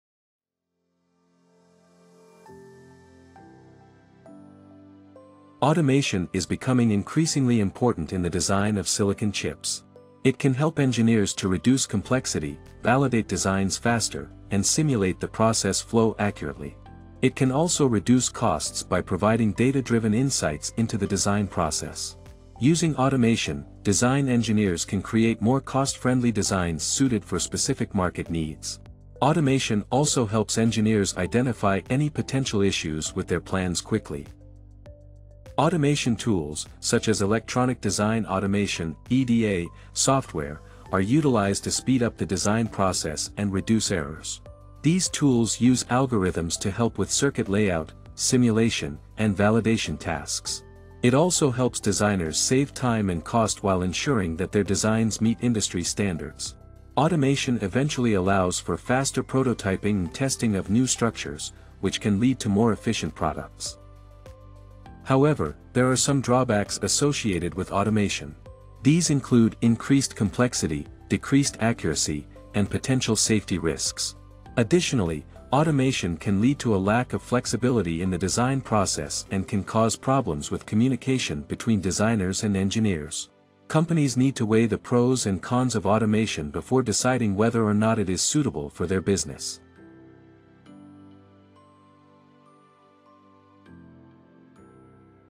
VLOG-201 | The Semiconductor Better Chips
1/ - Automation Is Becoming Increasingly Important In The Design Of Silicon Chips. It Can Help Engineers To Reduce Complexity, Validate Designs Faster, And Simulate The Process Flow Accurately. It Can Also Reduce Costs By Providing Data-Driven Insights Into The Design Process.

2/ - Using Automation, Design Engineers Can Create More Cost-Friendly Designs Suited For Specific Market Needs. Automation Also Helps Engineers Identify Any Potential Issues With Their Plans Quickly.

3/ - Automation Tools, Such As Electronic Design Automation (EDA) Software, Are Utilized To Speed Up The Design Process And Reduce Errors. These Tools Use Algorithms To Help With Circuit Layout, Simulation, And Validation Tasks.

4/ - It Also Helps Designers Save Time And Cost While Ensuring That Their Designs Meet Industry Standards. Automation Eventually Allows For Faster Prototyping And Testing Of New Structures, Which Can Lead To More Efficient Products.

5/ - However, There Are Some Drawbacks Associated With Automation. These Include Increased Complexity, Decreased Accuracy, And Potential Safety Risks. Additionally, Automation Can Lead To A Lack Of Flexibility In The Design Process And Can Cause Problems With Communication Between Designers And Engineers. Companies Need To Weigh The Pros And Cons Of Automation Before Deciding Whether Or Not It Is Suitable For Their Business.

ABOUT:

Chetan Arvind Patil's vlog for the less techy folks who use hardware, software, semiconductor and technology more than they might realize.